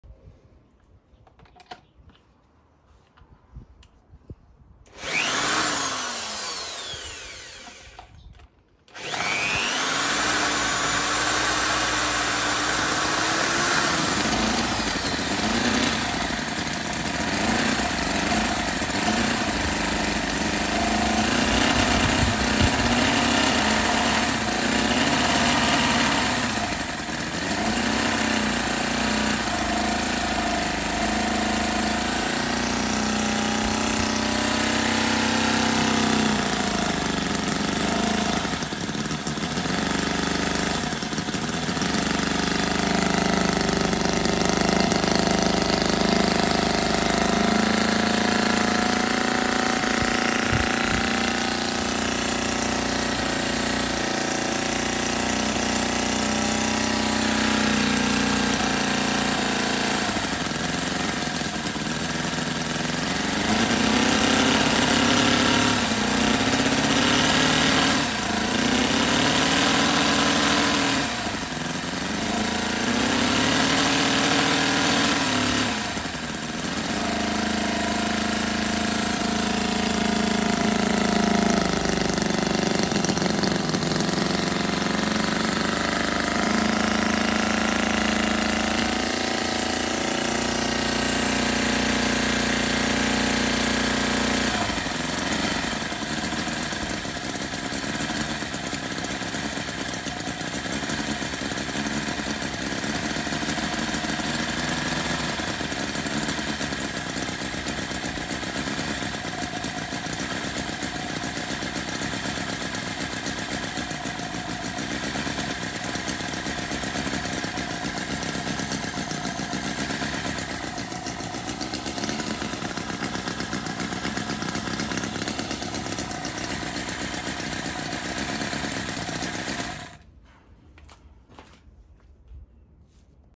Scale model Morris engine, again…a few more refinements.
I’ve replaced the home made carburettor with this carby from a model aeroplane engine. I now have better control over the rev range instead of it dying whenever I try to make any adjustments.

One of the four ignition coils that were hidden underneath the base died so I have now changed to a single unit and I had to make up a new distributor with a hall sensor built in. The distributor cap looks similar to what would have been on the original 1920’s engine and just by chance the wires that exit the top of the cap are in the correct firing order so they run directly to the plugs without getting all crossed up.

Also, it can’t be seen in the video but I’ve also installed a water tank in the base to increase the overall water capacity to help with cooling. ￼

The new exhaust is an added touch to help keep the engine frame and base a bit cleaner.